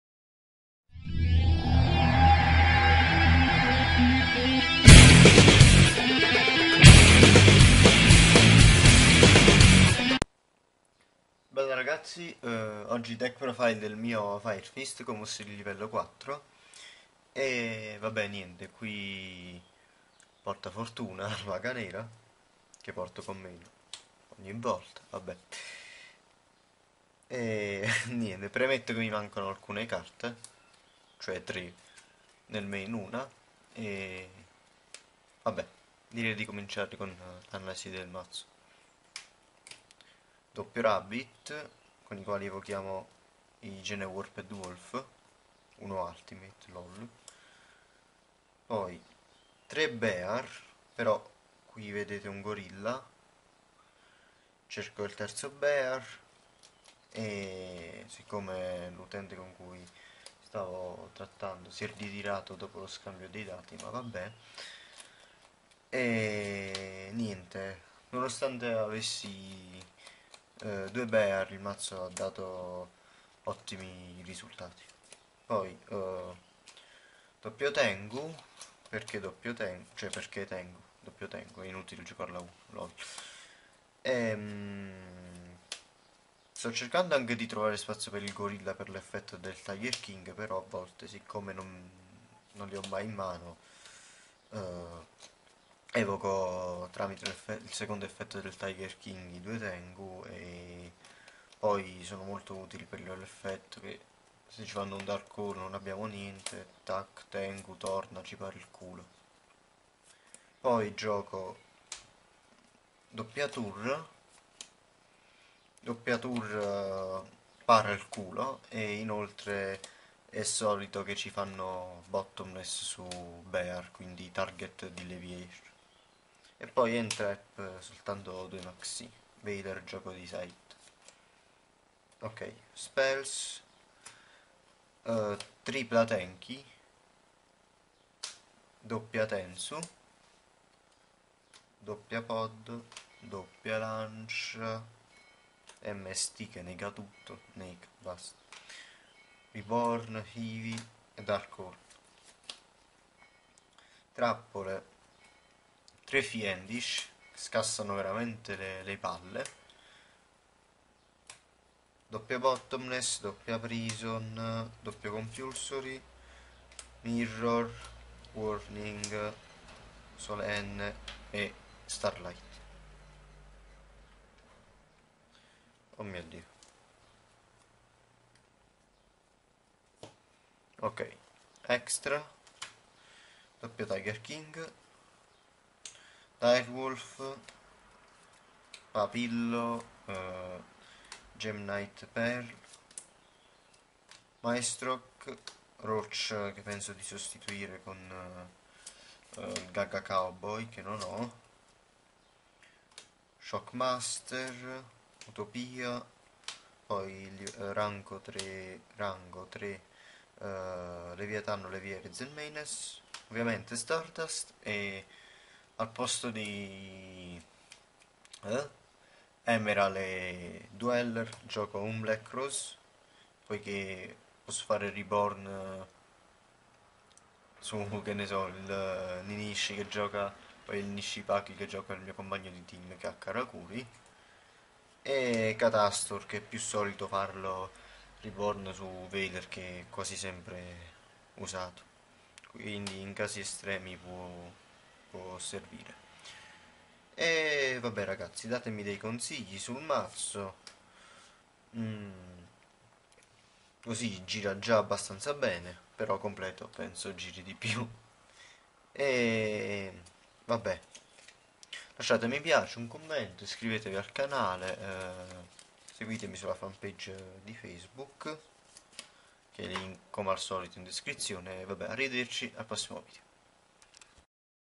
Yu-Gi-Oh! Deck Profile: Fire Fist LV 4 August 2013 (March 2013 Format)
96StardustDragon's Fire Fist Deck
Monsters [14]
2x Rescue Rabbit
3x Gene-Warped Wolf
3x Fire Fist - Bear
2x Reborn Tengu
2x Tour Guide from the Underworld
2x Maxx "C"
Spells [13]
3x Fire Formation - Tenki
2x Fire Formation - Tensu
2x Pot of Duality
2x Forbidden Lance
1x Heavy Storm
1x Dark Hole
1x Monster Reborn
1x Mystical Space Typhoon
Traps [13]
3x Fiendish Chain
2x Bottomless Trap Hole
2x Dimensional Prison
2x Compulsory Evacuation Device
1x Mirror Force
1x Starlight Road
1x Solemn Warning
1x Solemn Judgment